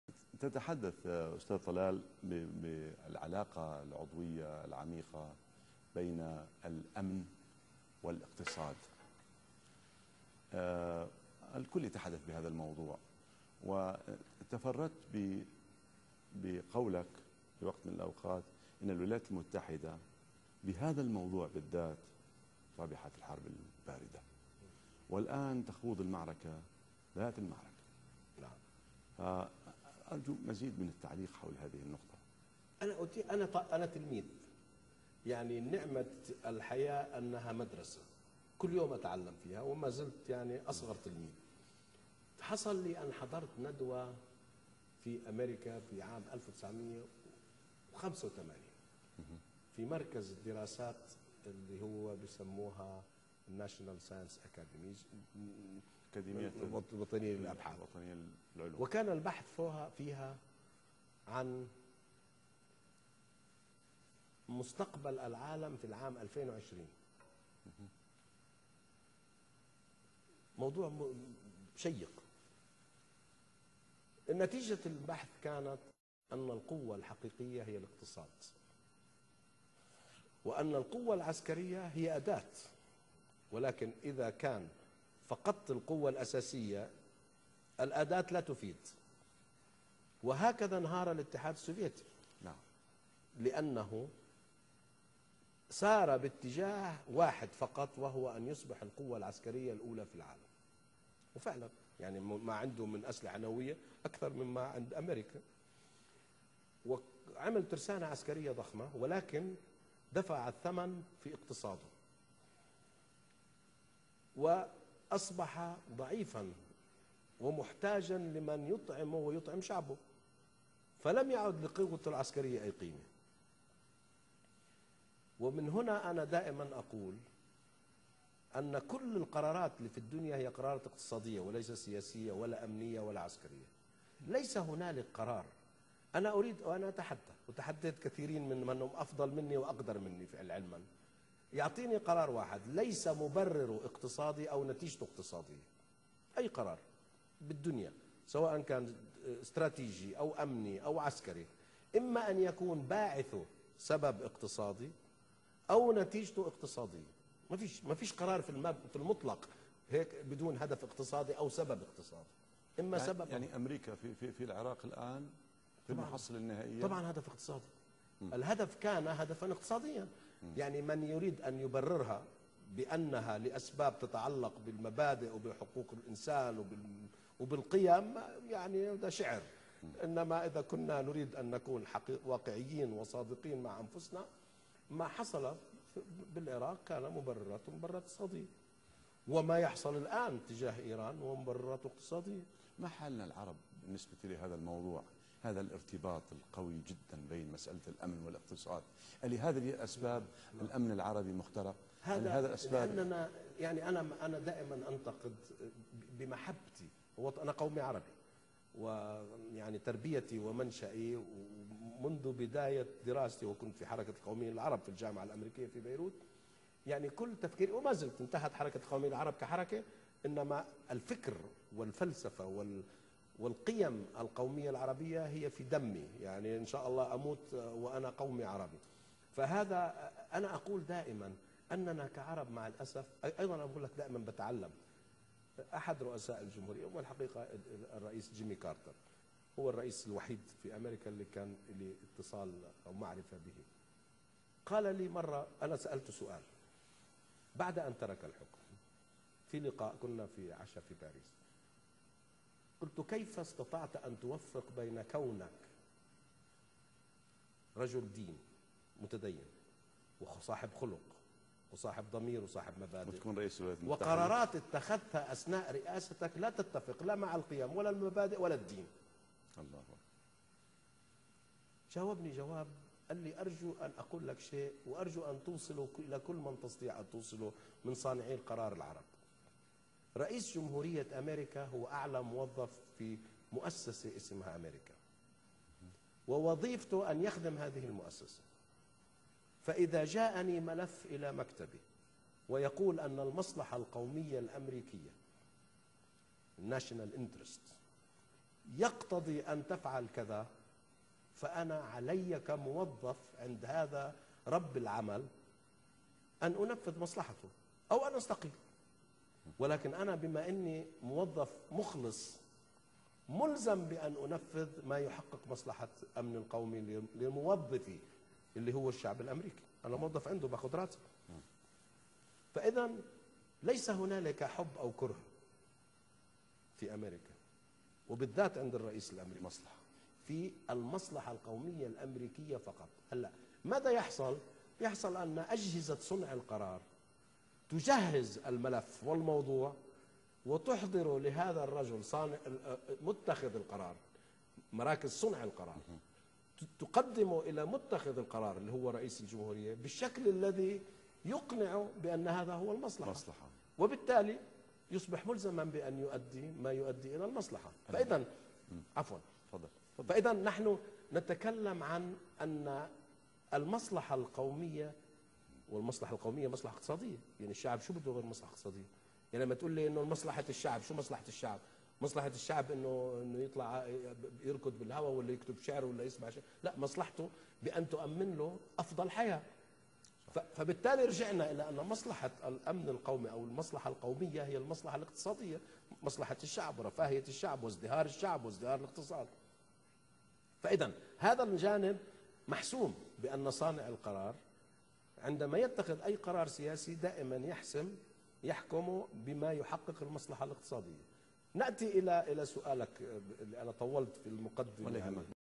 Talal Abu-Ghazaleh's interview with Alrai channel 3A/6
Alrai satellite channel interviews Mr. Talal Abu-Ghazaleh.